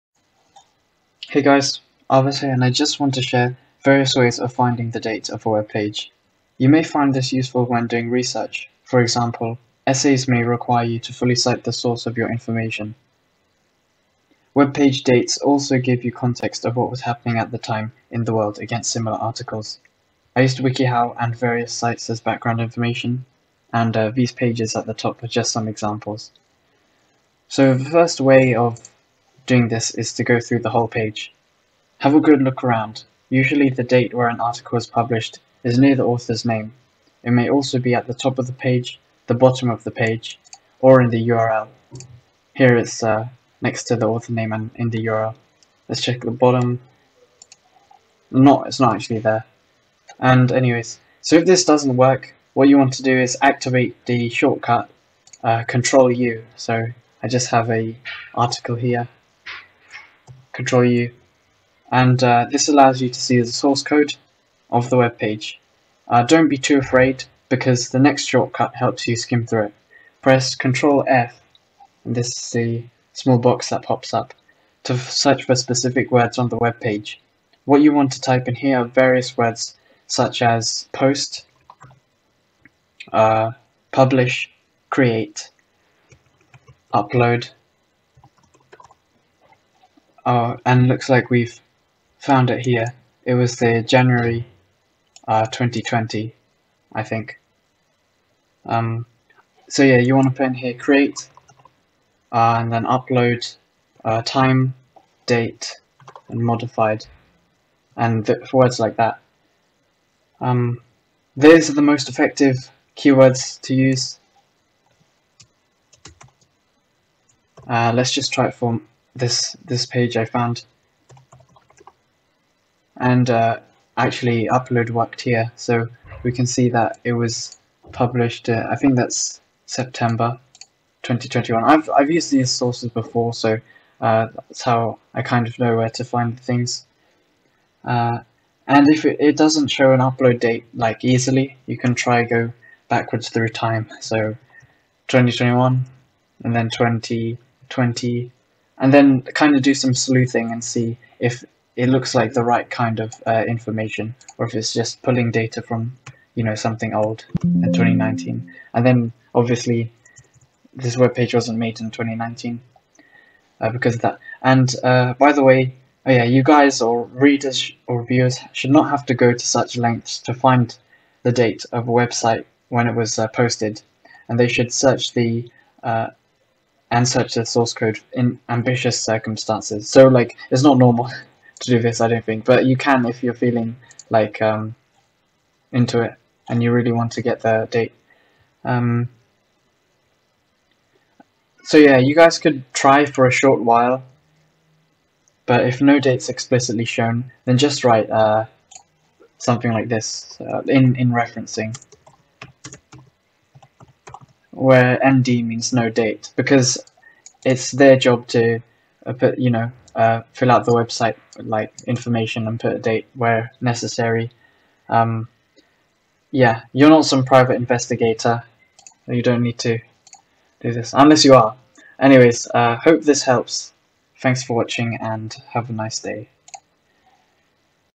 How to Find the Date of a Web Article
Hey guys, I wanted to share a couple of ways to find the date when a web article was published. It may come in handy when referencing online articles which is what I use these techniques for. You'll need to use the Ctrl+U and Ctrl+F shortcuts for the advanced method, and other than that it's fairly simple to search for. Use specific words in that search box like post/ publish/ create/ upload/ time/ date/ modified as these are the most effective in searching the page for that upload date. Hope it helps!

If these methods fail and it is not clear what the upload date is, just put (n.d.) in referencing, since the article does not include it.